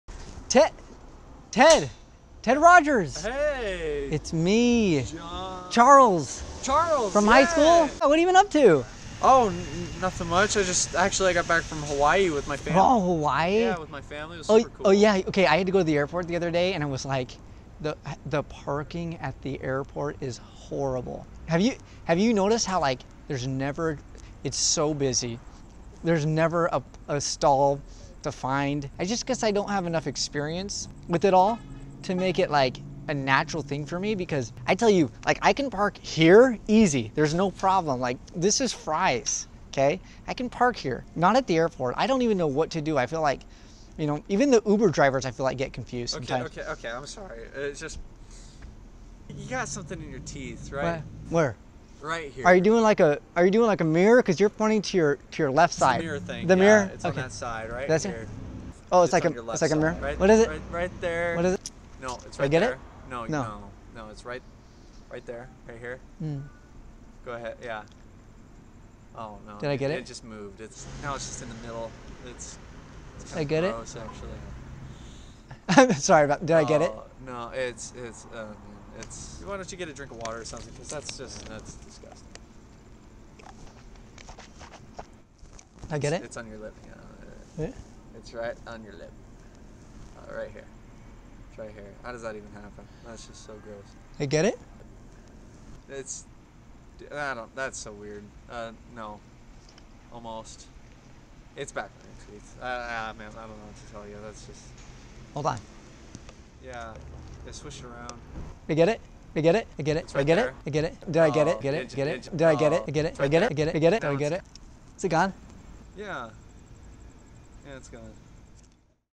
Tooth Berry
Charlie has something in his teeth.